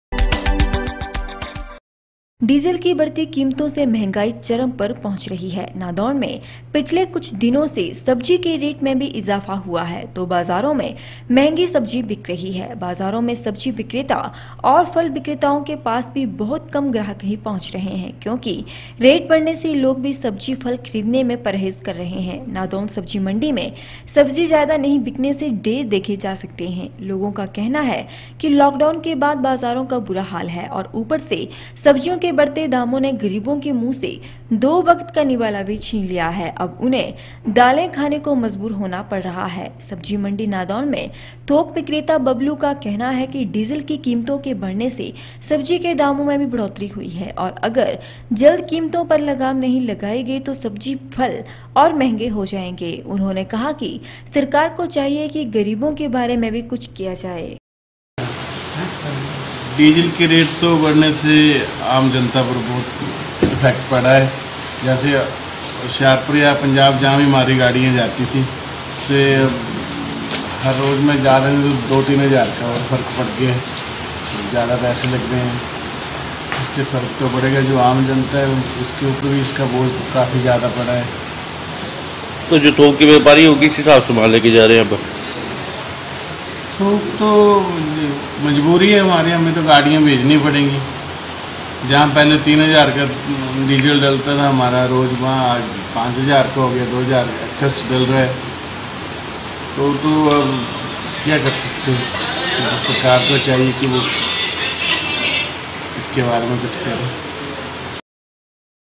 17 july 13 डीजल की बेहताशा बढती कीमतों से सब्जी के रेट सातवें आसमान पर ।
ऐंकर नादौन डीजल की बेहताशा बढती कीमतों से सब्जी के रेट सातवें आसमान पर ।
बीओ-बाईट डीजल की बढती कीमतों से महंगाई चरम पर पहुंच रही है । नादौन में पिछले कुछ दिनों से सब्जी के रेट में भी इजाफा हुआ है तो बाजारों में महंगी सब्जी बिक रही है। बाजारों में सब्जी विक्रेता और फल बिक्रेताओं के पास भी बहुत कम ग्राहक पहुंच रहे है क्योंकि रेट बढने से लोग भी सब्जी फल खरीदने में परहेज कर रहे है। नादौन सब्जी मंडी में सब्जी ज्यादा नहीं बिकने से ढेर देखे जा सकते है।